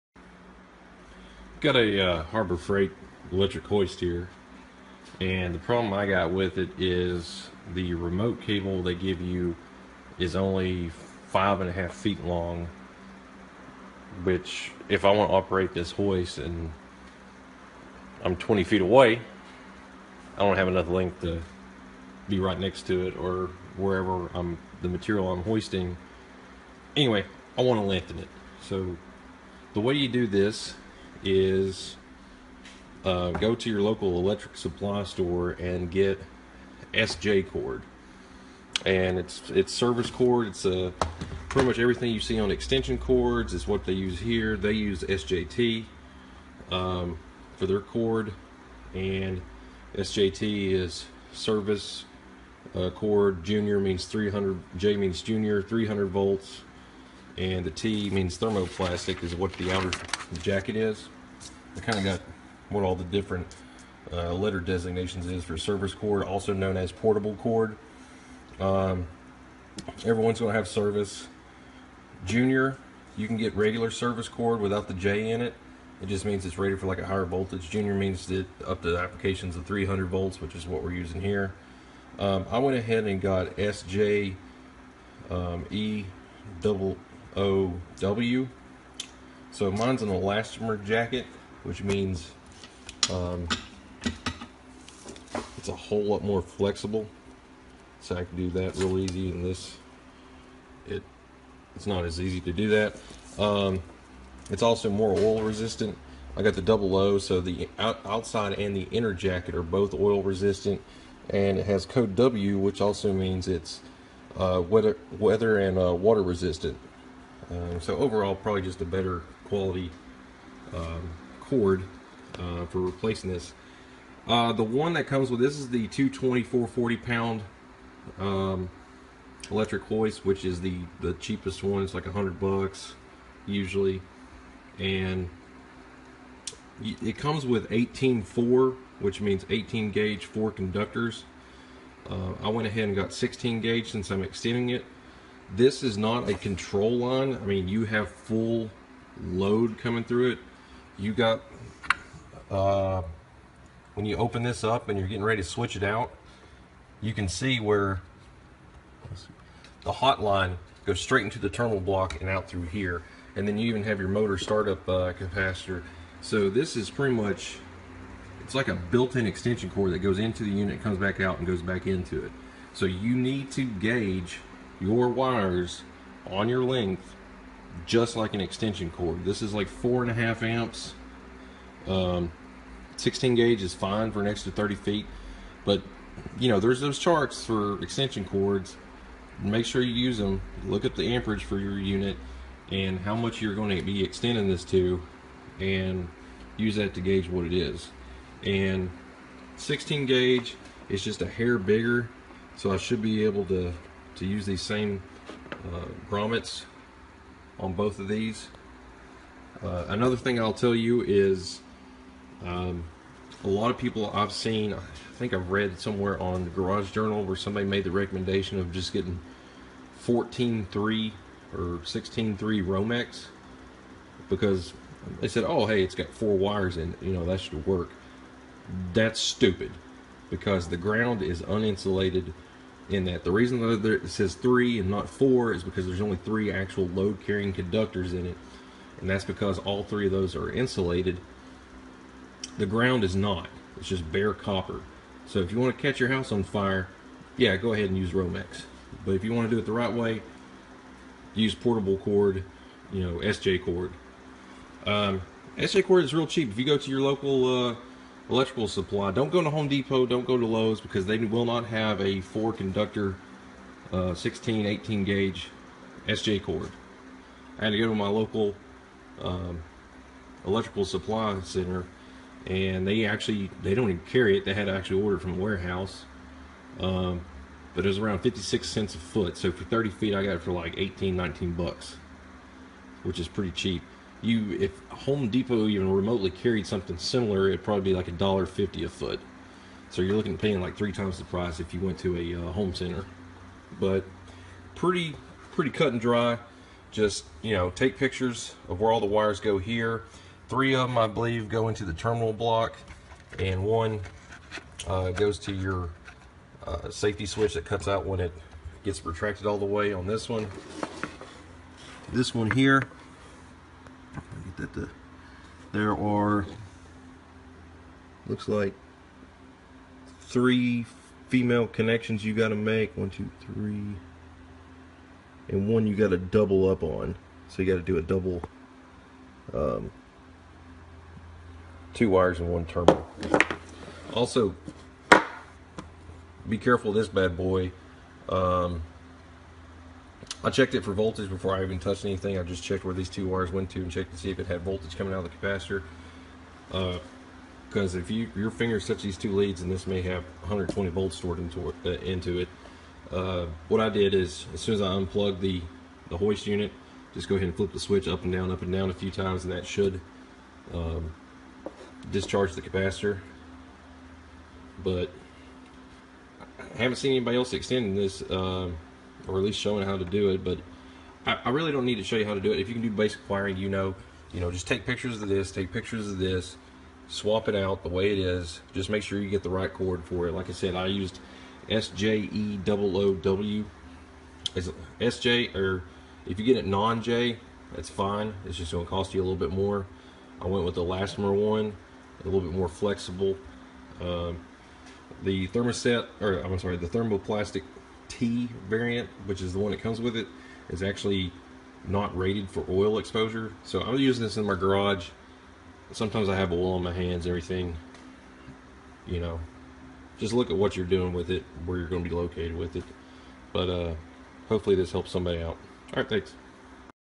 Hướng dẫn kéo dài tay điều khiển của tời điện mini
Tời kéo mini chạy điện 1pha là loại máy tời được sử dụng khá phổ biến hiện nay. Tơi được sử dụng bằng cách treo 1 điểm cố định trên cao sau đó điều khiển nâng hạ hàng hóa thông qua 1 tay điều khiển có các nút nên xuống, nút tạm dừng.
Theo thiết kế, chiều dài tiêu chuẩn của tay điều khiển này chỉ từ 3 - 5met. vì vậy nhiều khi khách hàng muốn treo tời lên cao hơn thì khá bất tiện. Vậy làm sao để kéo dài được tay điều khiển này mời các bạn xem trong clip này.
Nếu bạn có nhu cầu mua các sản phẩm máy tời điện hãy đến với Thăng Long Group để được tư vấn, báo giá và cung cấp sản phẩm chính hãng.